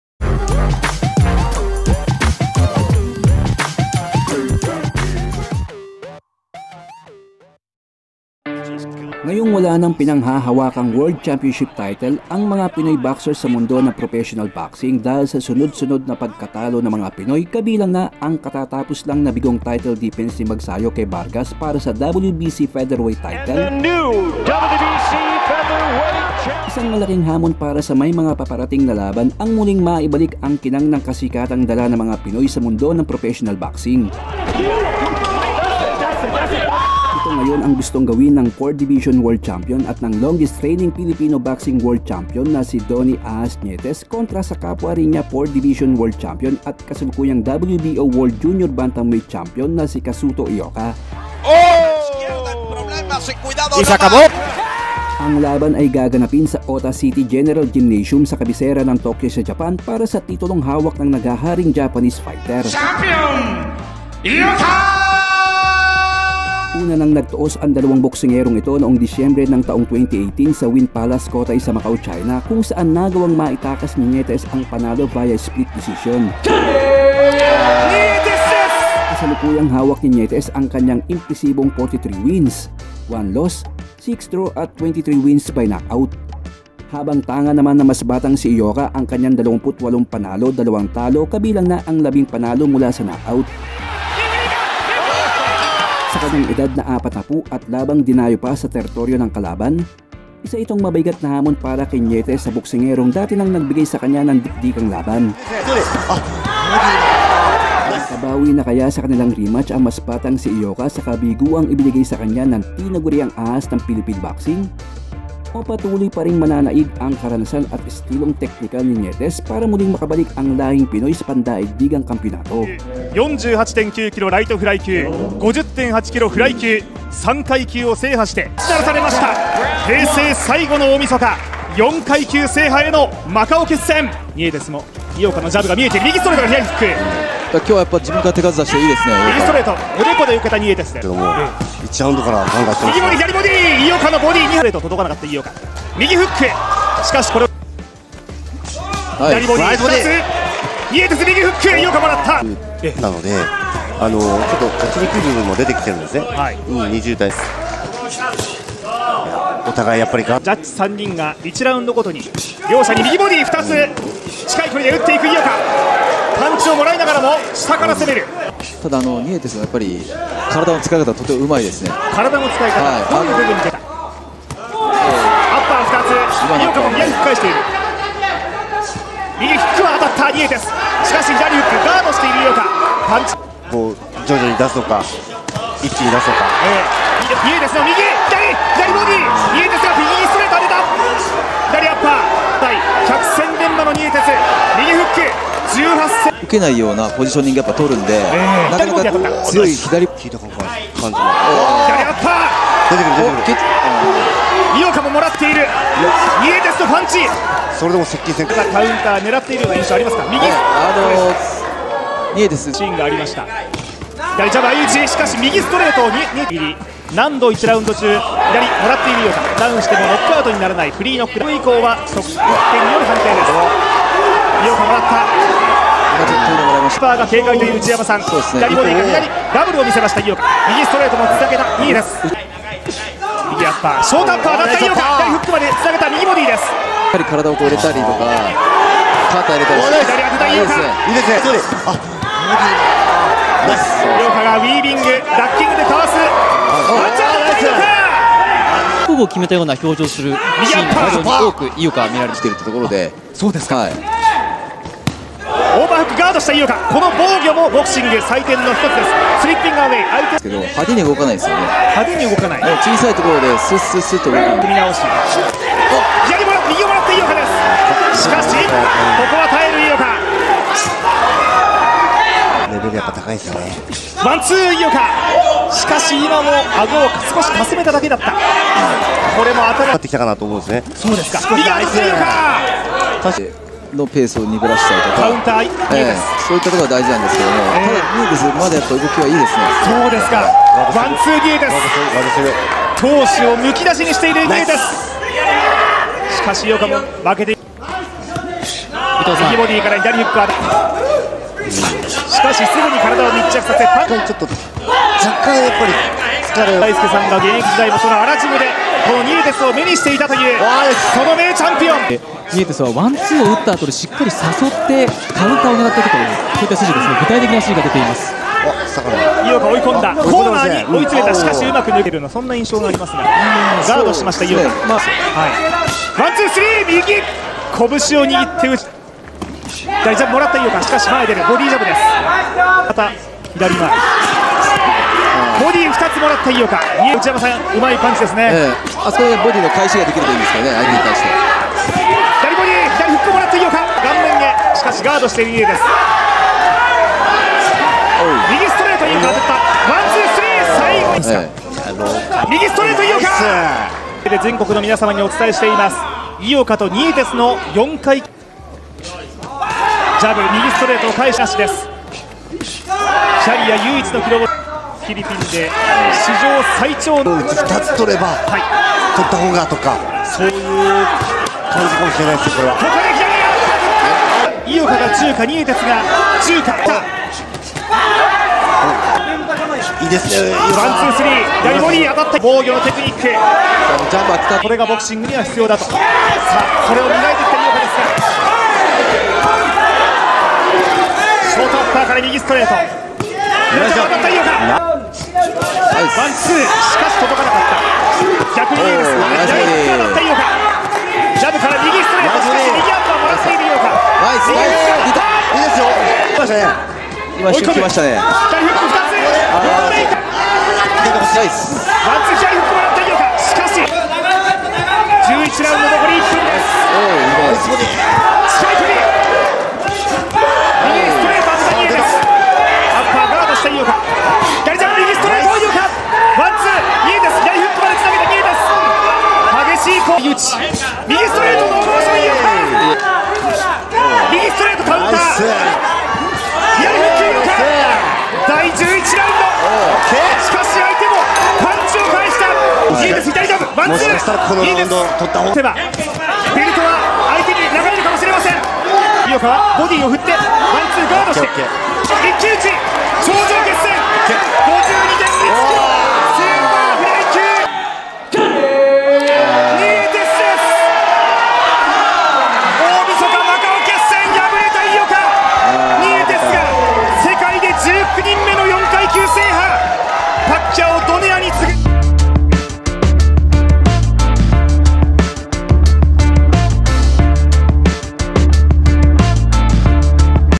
DONNIE NIETES vs. KAZUTO IOKA FIGHT
Donnie "AHAS" Nietes a 4 Division World Champion and the Longest-Reigning Filipino World Champion will face Japan:s only 4 Division World Champion and current WBO World Junior Bantamweight Champion Kazuto Ioka for thier rematch that will take palce at Ota City General Gymnasium Tokyo Japan this July 13, 2022.

While Nietes got the win in the first meeting, Ioka has come strong since then and he's ready to avenge that loss. At 40 years of age can Nietes find a way to duplicate that win again?